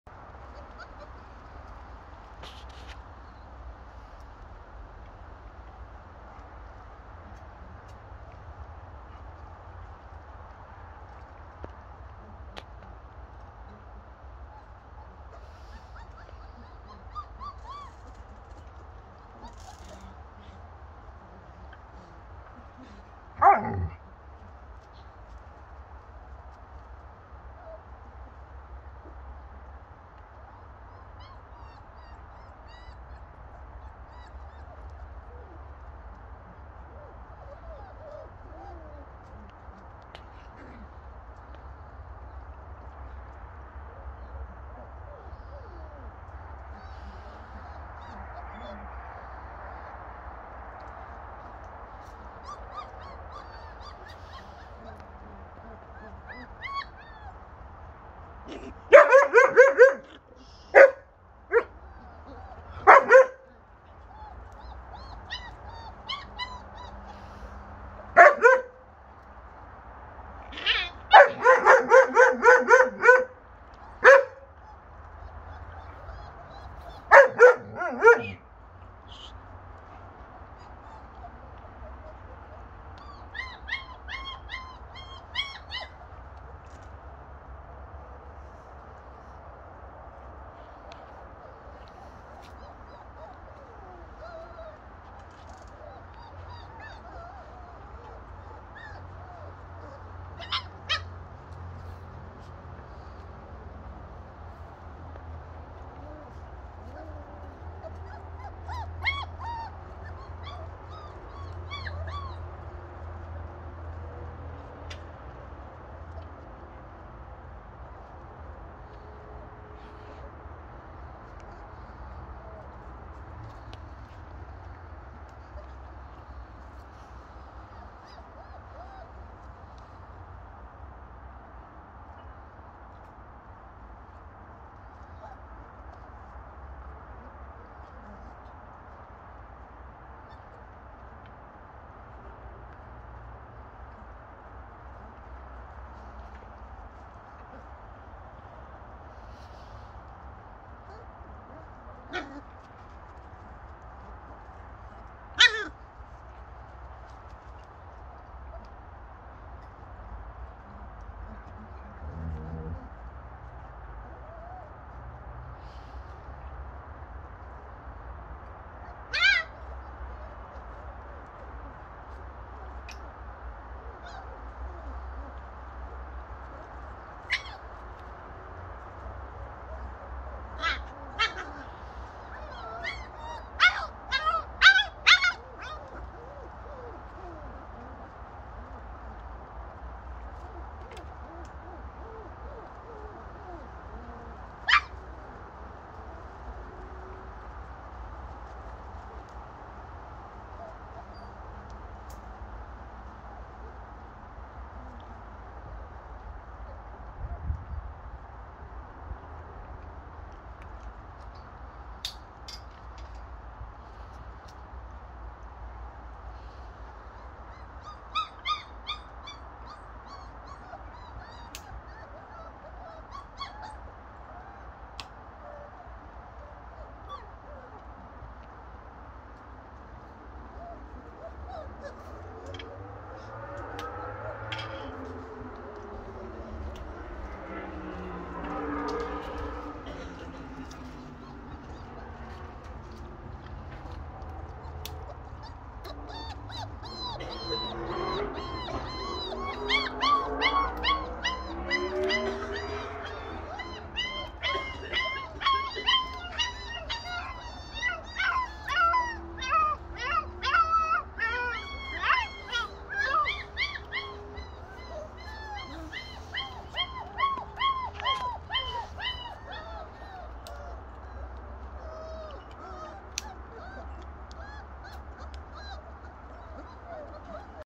German shepherd mum (and nanny) playing with litter of 7 puppies in our garden London
21st of June, we had a beautiful and healthy black German Shepherd litter of 7 puppies.